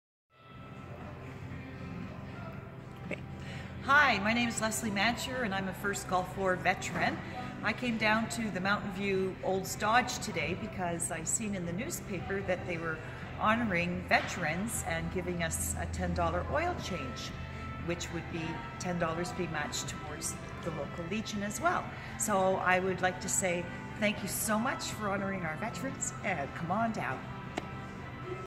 Veteran | $10 Oil Change
Leslie was in for a $10 Oil Change.

32580 11 Range Road #43
Olds, AB
T4H 1P6